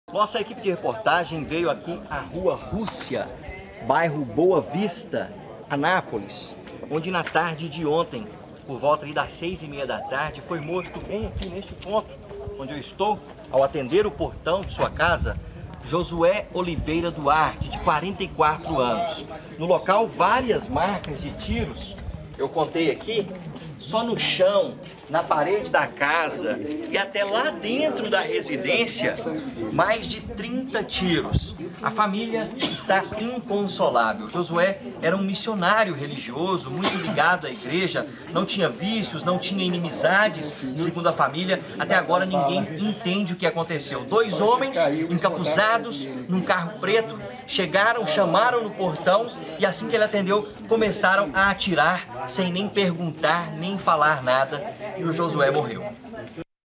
CRIME MISTERIOSO: MISSIONÁRIO É EXECUTADO A TIROS
Um missionário morador de Anápolis foi executado com cerca de onze tiros na casa onde ele morava. A família não consegue entender a motivação do crime.